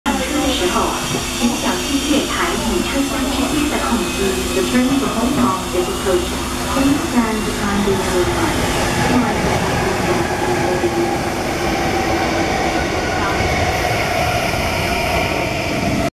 列車24進入上水月台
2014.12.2.上水月台 12:42 列車24號住紅磡方向埋站